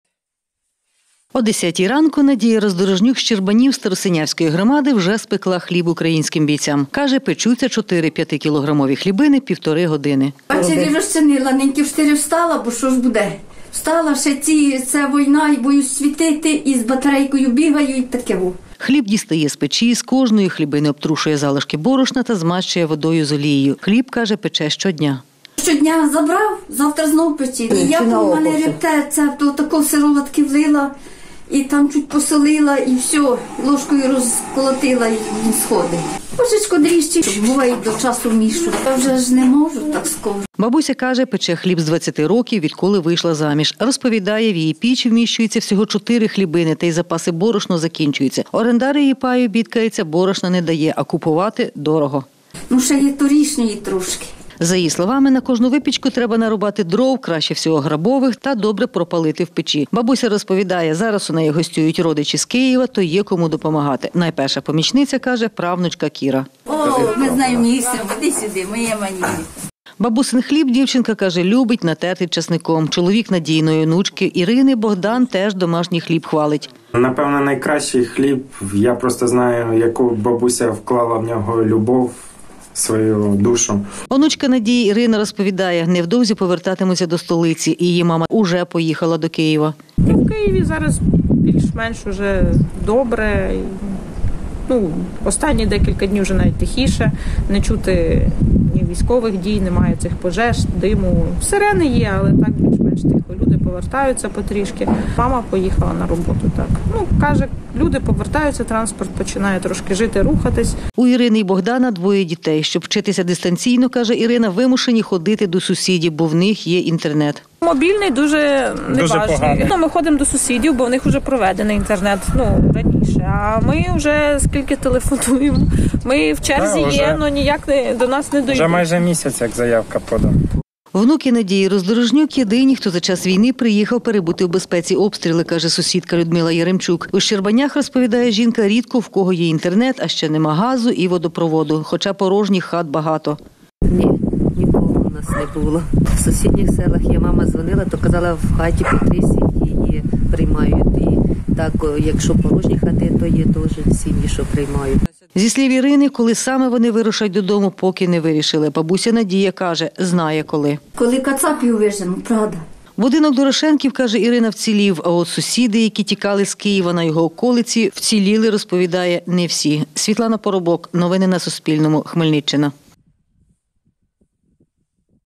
75-річна селянка з Хмельниччини пече хліб для військових української армії
Надія Роздорожнюк з села Щербані Старосинявської громади щодня пече для бійців Збройних сил України піч хліба та передає випічку волонтерами. Бабуся розповідає: пекла б і більше, якби орендарі давали за її пай борошно. Із 75-річною волонтеркою зустрілися кореспонденти Суспільного.